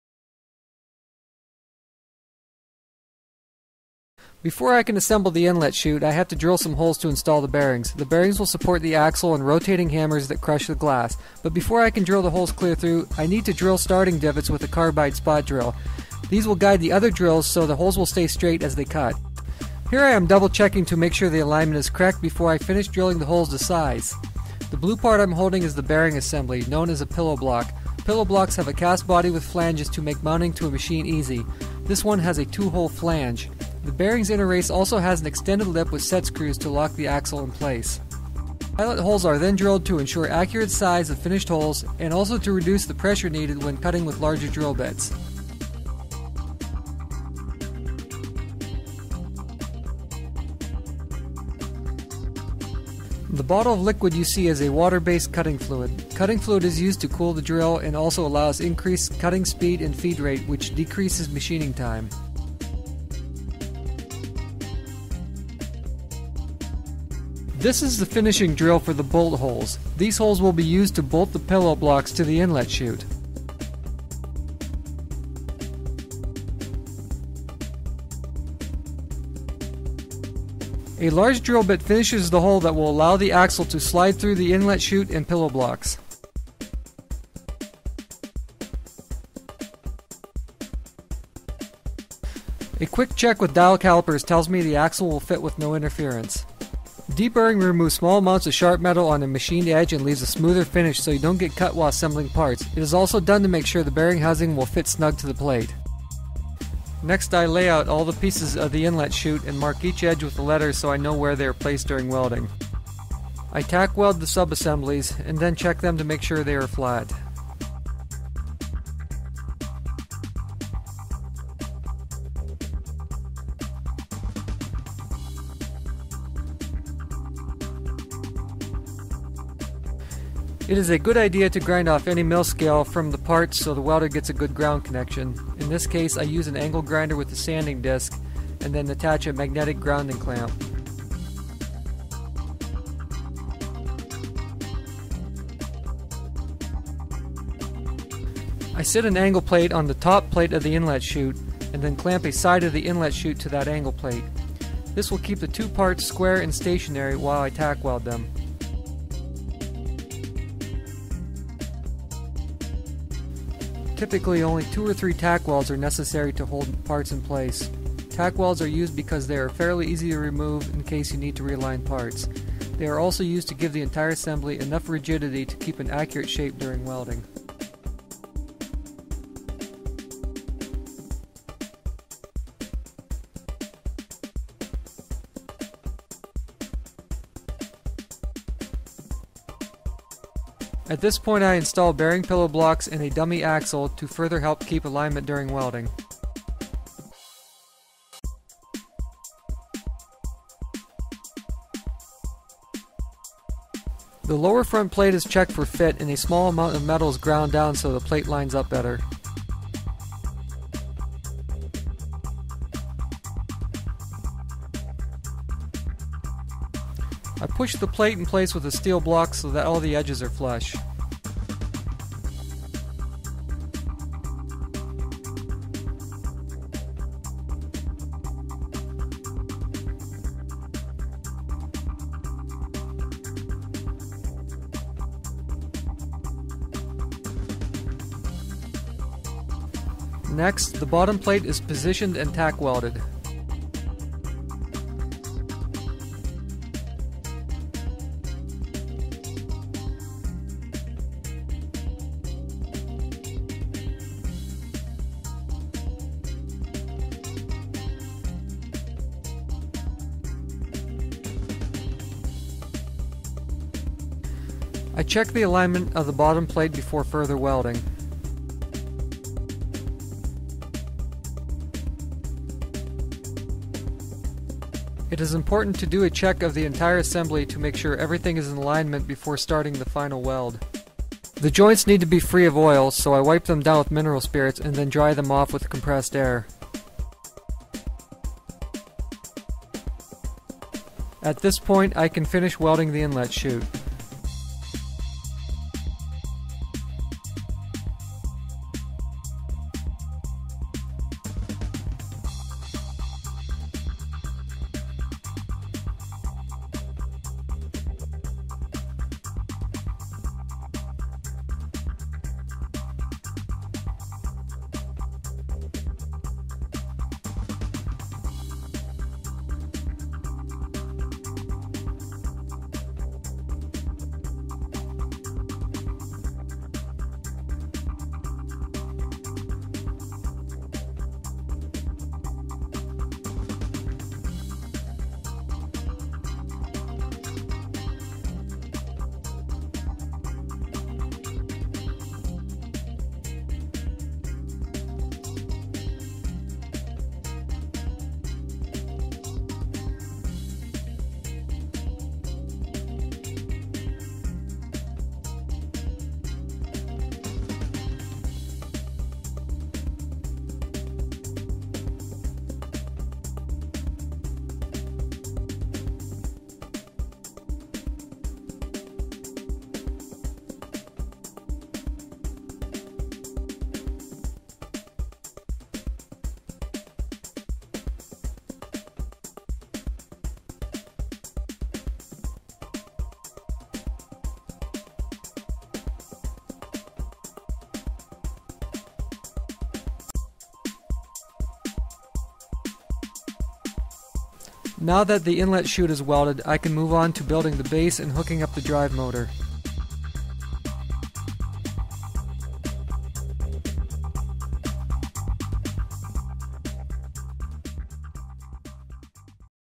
Building My Second Glass Crusher Part 2 of 7
This is part 2 of the fabrication of my second glass crusher / hammer mill design.

Narration transcript:
Before I can assemble the inlet chute, I have to drill some holes to install the bearings. The bearings will support the rotating hammers that crush the glass. Before I can holes clear through, I need to drill starting divots with a carbide spot drill. These will guide the other drills so the holes will stay straight during drilling.

Here I am double checking to make sure the alignment is correct before I finish drilling the holes to size. The blue part I'm holding is the bearing assembly, known as a pillow block. Pillow blocks have a cast body with flanges to make mounting to a machine easy. This one is has a two hole flange. The bearing's inner race also has an extended lip with set screws to lock the axle in place.

Pilot holes are then drilled to ensure accurate size of finished holes, and also to reduce the pressure needed when cutting with larger drill bits.

The bottle of liquid you see is a water based cutting fluid. Cutting fluid is used to cool the drill, and also allows increased cutting speed and feed rate, which decreases machining time.

This is the finishing drill for the bolt holes. These holes will be used to bolt the pillow blocks to the inlet chute.

A large drill bit finishes the hole that will allow the axle to slide through the inlet chute and pillow blocks.

A quick check with dial calipers tells me the axle will fit with no interference.

Deburring removes small amounts of sharp metal on a machined edge, and leaves a smoother finish so you don't get cut while assembling parts. It is also done to make sure the bearing housing will fit snug to the plate.

Next I layout all the pieces of the inlet chute and mark each edge with a letter so I know where they are placed during welding.

I tack weld the sub assemblies, and then check them to make sure they are flat.

It is a good idea to grind off any mill scale from the parts so the welder gets a good ground connection. In this case, I use an angle grinder with a sanding disc, and then attach a magnetic grounding clamp.

I sit an angle plate on the top plate of the inlet chute, and then clamp a side of the inlet chute to that angle plate. This will keep the two parts square and stationary while I tack weld them.

Typically, only two or three tack welds are necessary to hold parts in place. Tack welds are used because they are fairly easy to remove in case you need to realign parts. They are also used to give the entire assembly enough rigidity to keep an accurate shape during welding.

At this point I install the bearing pillow blocks and a dummy axle to further help keep alignment during welding.

The lower front plate is checked for fit and a small amount of metal is ground down so the plate lines up better.

I push the plate in place with a steel block so that all the edges are flush.

Next, the bottom plate is positioned and tack welded.

I check the alignment of the bottom plate before further welding.

It is important to do a check of the entire assembly to make sure everything is in alignment before starting the final weld.

The joints need to be free of oil, so I wipe them down with mineral spirits, and then dry them off with an compressed air.

At this point, I can finish welding the inlet chute.

Now that the inlet chute is welded, I can move on to building the base and hooking up the drive motor.